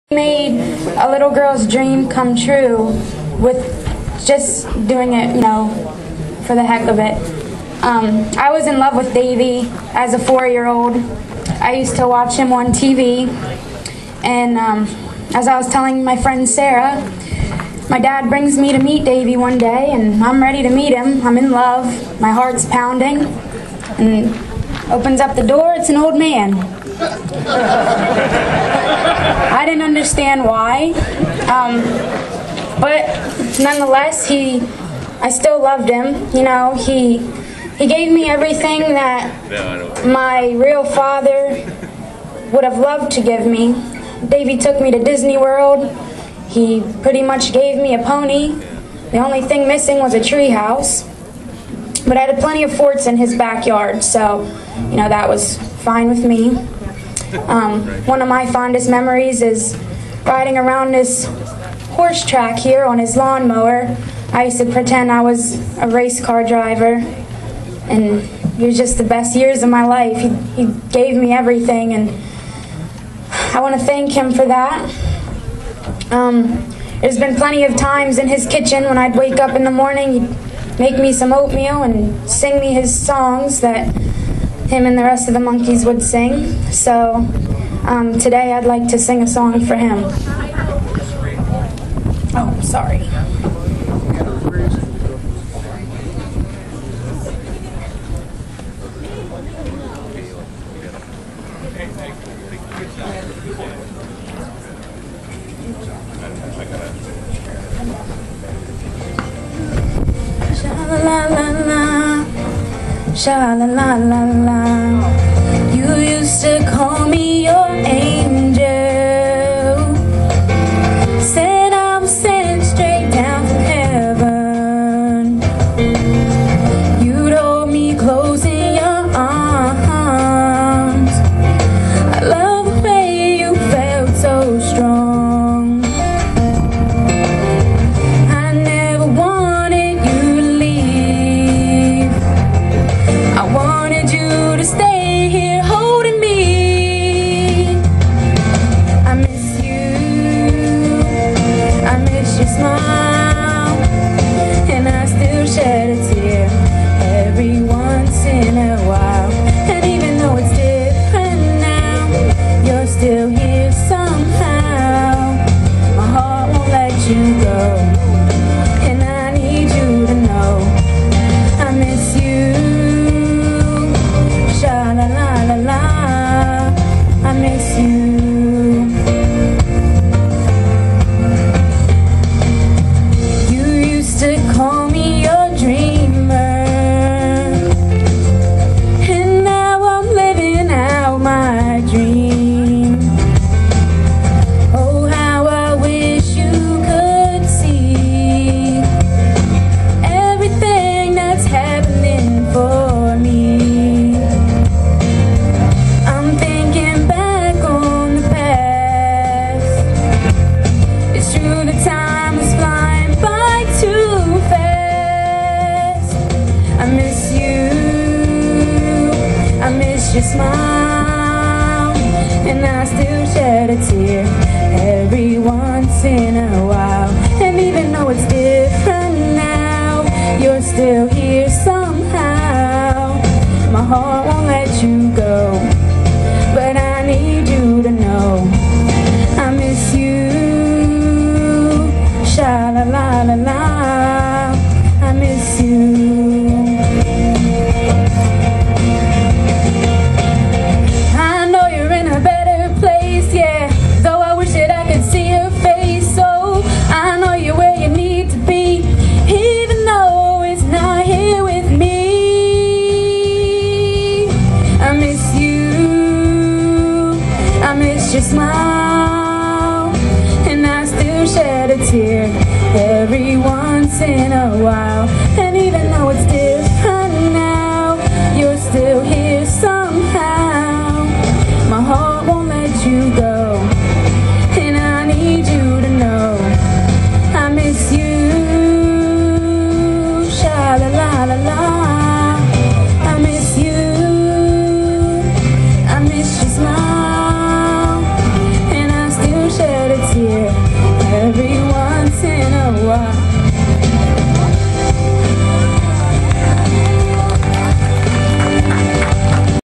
Davy Jones Beavertown Memorial Part 3
Public memorial service held in honor of Davy Jones in Beavertown Pennsylvania. This video was recorded with permission by Sarah Combs and Jeff Calfee and has been made publicly available free of charge with permission of the Jones Family. This video is not to be reproduced or sold for profit.